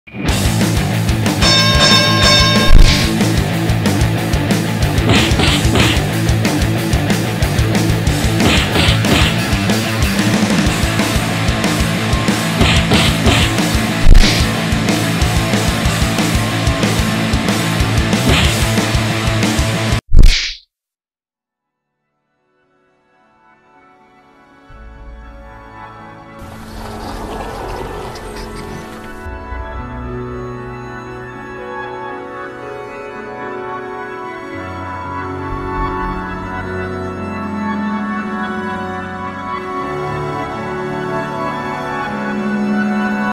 Mario vs The Kongs!!!
Everyone’s favourite Italian plumber, Mario, faces off against the Kongs who have kidnapped Princess Peach. Mario is now Peach’s only hope, and will he be able to defeat all three of the Kong family (Donkey, Diddy and Dixie Kong) or will they ultimately defeat him and kill Peach? Find out now…

This was done as part of my college work for Games & Animation Level 1.
List of effects I used (raw files to the right of the name and link):

- Boxing Punches, recorded by Mike Koenig (Boxing Punches-SoundBible.com-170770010.mp3)

- Strong Punch, recorded by Mike Koenig (Strong_Punch-Mike_Koenig-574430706.mp3)

- Boxing Arena Sound, recorded by Samantha Enrico (Boxing_arena_sound-Samantha_Enrico-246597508.mp3)

- Sliding Door, recorded by ezwa (Sliding Door-SoundBible.com-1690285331.mp3)